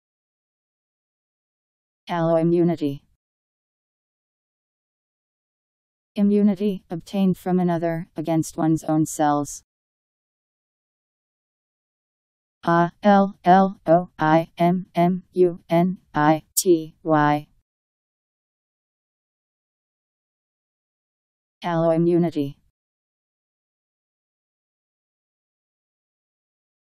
Alloimmunity Meaning
Video shows what alloimmunity means. Immunity, obtained from another, against one's own cells..  Alloimmunity Meaning. How to pronounce, definition audio dictionary. How to say alloimmunity. Powered by MaryTTS, Wiktionary